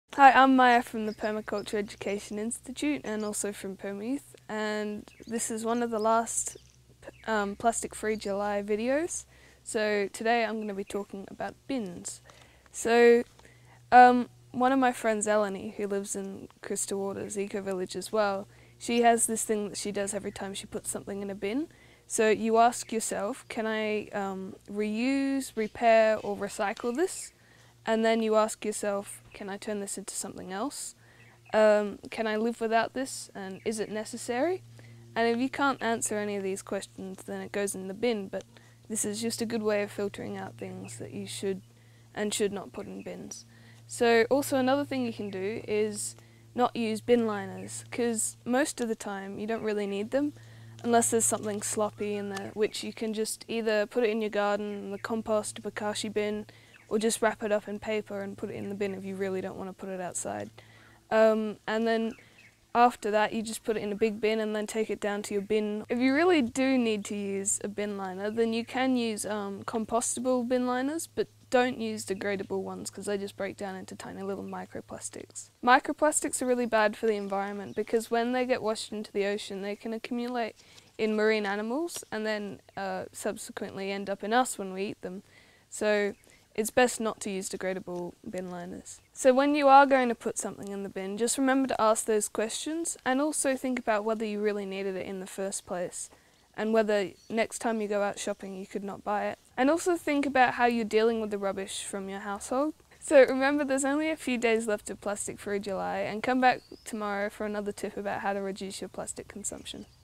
How to reduce plastic with your bins & bin bags - ft Maia Raymond | Day 29 Plastic Free July
My daughter, Maia, co-founder of the Permayouth talks today about one of the ideas they've been talking about in the Permayouth weekly online gatherings ... What can you do with the waste that is created in your home?

How about ditching the bin bag unless absolutely necessary, and then choosing a compostable (not degradable, because these often aren't as good as you think) bag.

Maia and the Permayouth, invite young people (11-17yo) who want to learn about permaculture and sustainable ways of living to join in. Check out the Permayouth website and get involved

What have been your best tips for cutting the waste that ends up in your bin?

Each day throughout Plastic Free July, we've been suggesting simple tips.